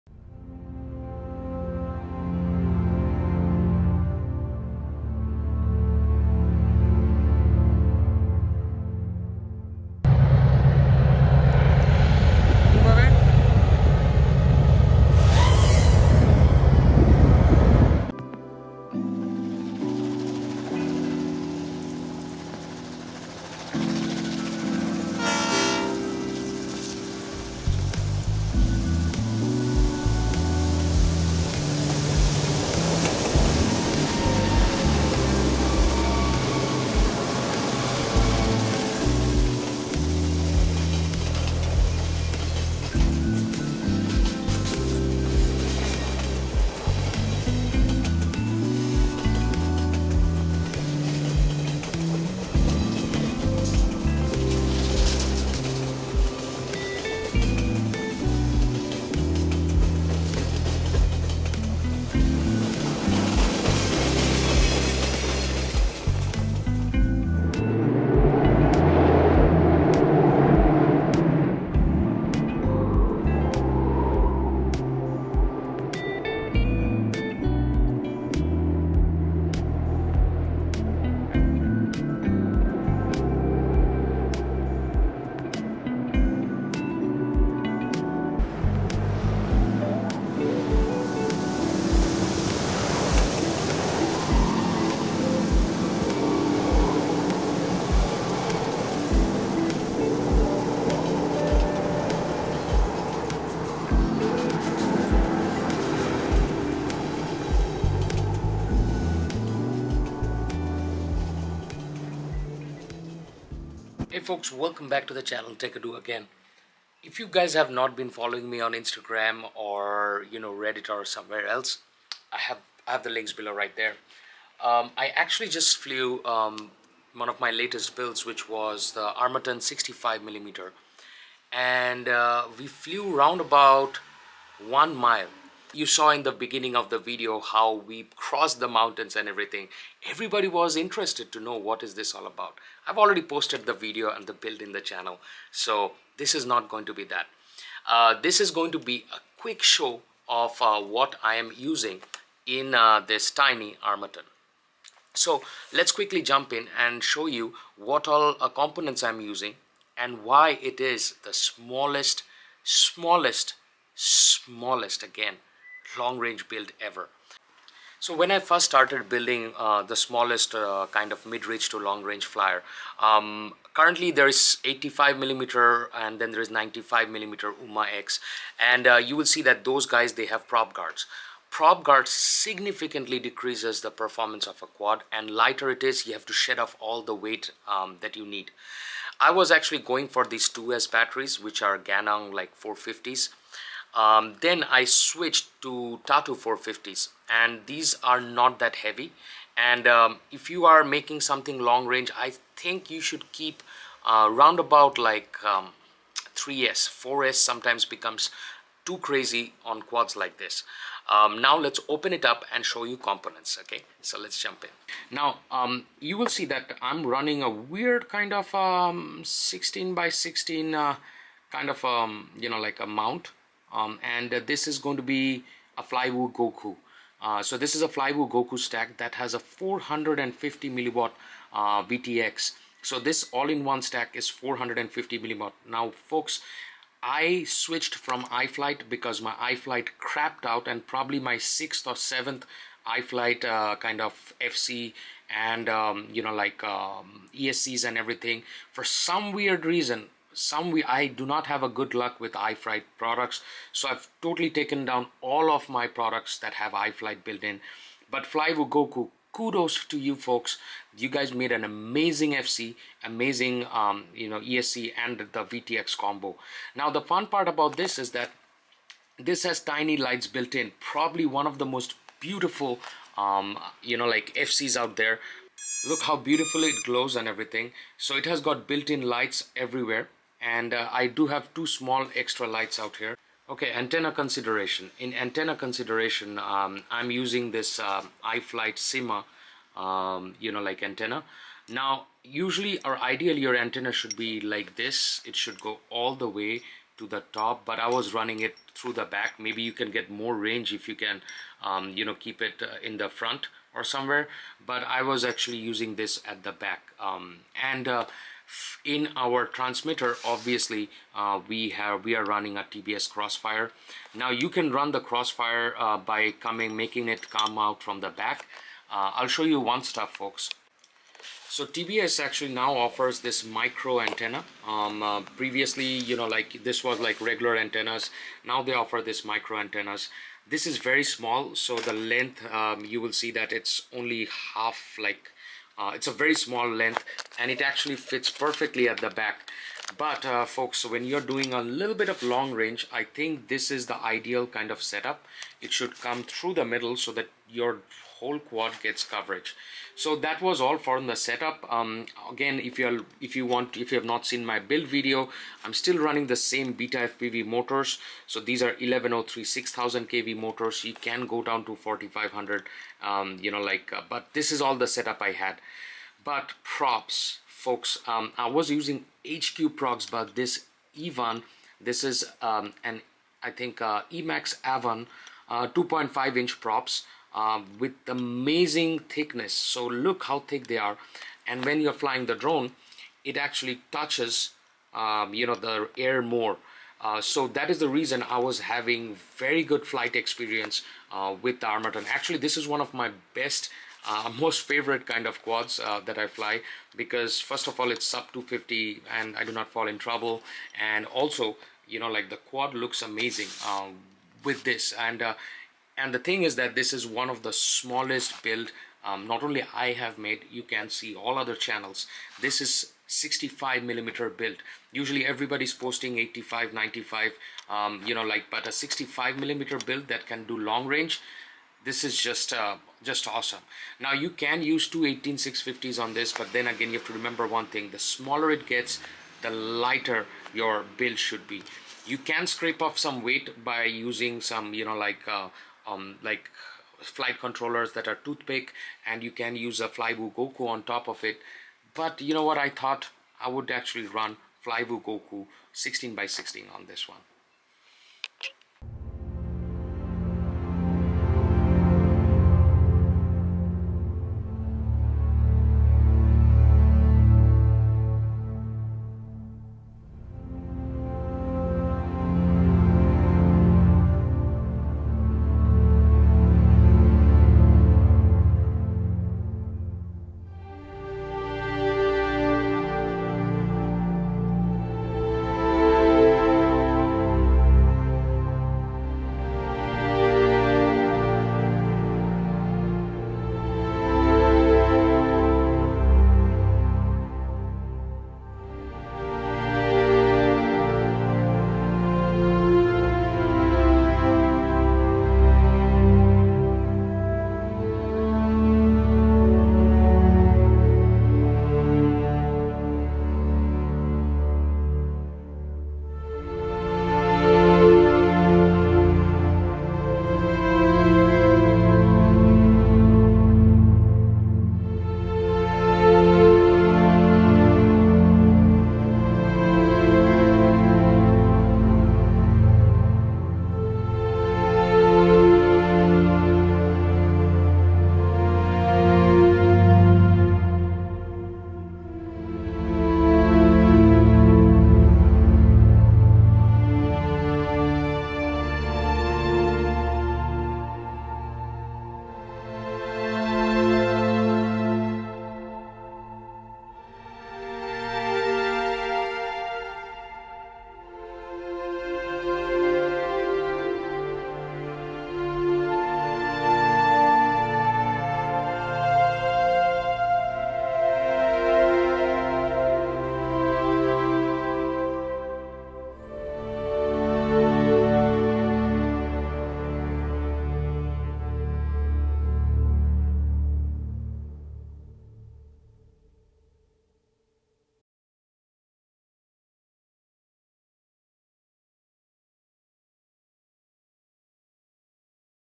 SMALLEST LONG RANGE 65 mm FPV DRONE ,test,footage,showdown.
2 SHORT FILMS ARE INLUDED IN THIS VIDEO
1. TOY TRAIN- 0:17
2. CALL OF NATURE - 7:27

links for the build most of them are from banggood
16*16 flywoo goku f411 micro stack
flywoo goku vtx
radiomaster crossfire nano and receiver bundle that i use on all of my quads
runcam nano

to put a naked gopro . please look into other youtube channels
as decasing of a gopro takes skills and also decreases longeivity of the product .

notes for the test
hi folks , smallest long range build for fpv drones , is now have become a thing. the change happened with flywoo bringing in 16*16 aio stack that has 450mw vtx , you definitely can go way past 1 mile , and in future i have a plan to do so to check the flight range way past 1 mile , this flight was test 4 as test 3 which was over water we had a limited range about 0.4 mile in distance.
Naked gopro is ideal to take cinematic footage in a small wind like this, but  after seeing all that hassle and it stopping work after few flights , i decided to go ahead and use the insta 360 go.
interference was noted while crossing the mountain and gps was not installed due to weight . we had laser range finders, with which flight distance was calculated with.